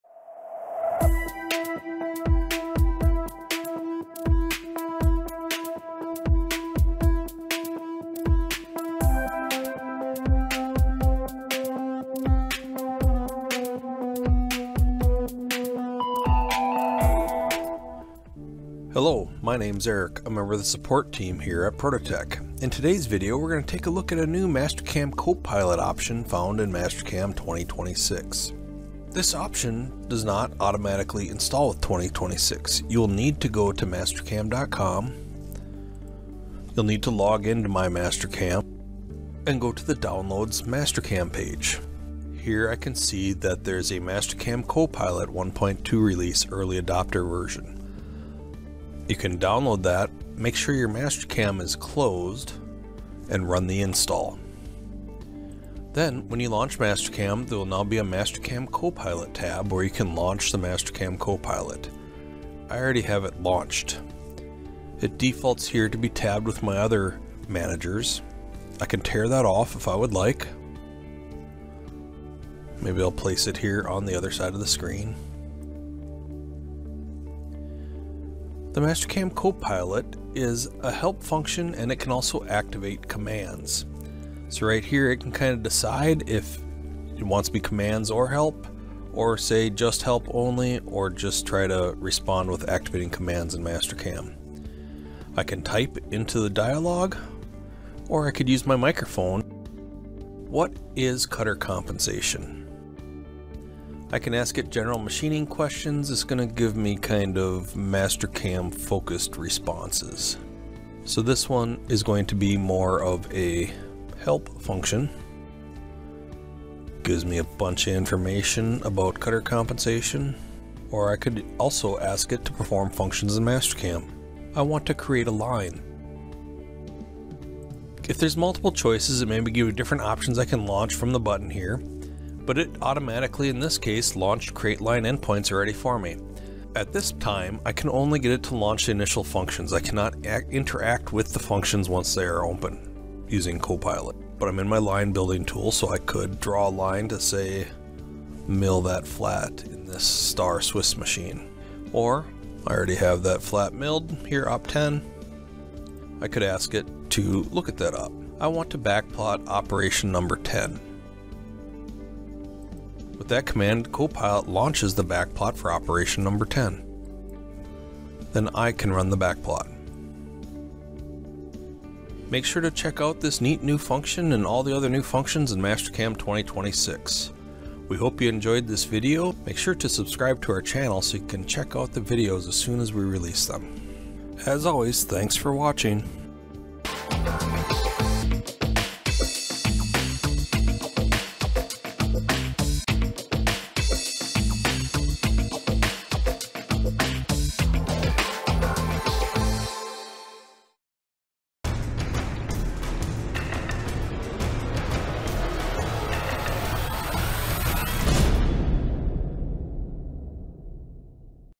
Mastercam AI Copilot Overview
ProtoTek presents various COPILOT features found in Mastercam 2026